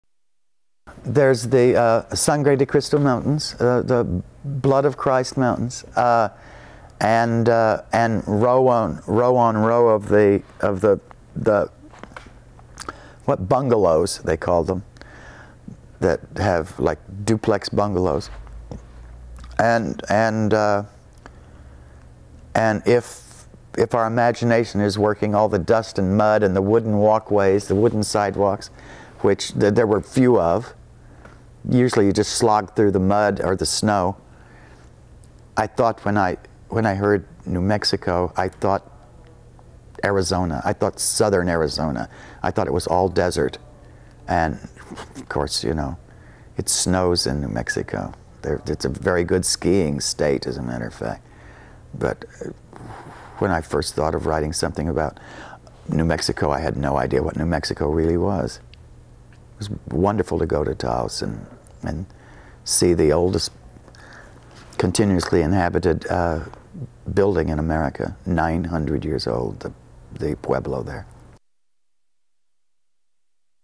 Lanford Wilson: Rain Dance: The setting
Playwright Lanford Wilson was raised by his mother in Lebanon, Missouri. He was born August 13, 1937 and died March 24, 2011.  In 1969, he co-founded the Circle Repertory Theatre in New York, with director Marshall Mason. His plays included Hot L Baltimore, Fifth of July, Talley's Folly, Balm in Gilead, Lemon Sky, The Mound Builders, Book of Days, Burn This, and Rain Dance. He won the Pulitzer Prize in 1980 for Talley's Folly. was interviewed by Mike Wood in January of 2001 at Jeff Daniels' Purpose Rose Theatre in Chelsea, Michigan and again in March, in St. Louis, Missouri. The interview segments are courtesy of the William Inge Center for the Arts in Independence, Kansas.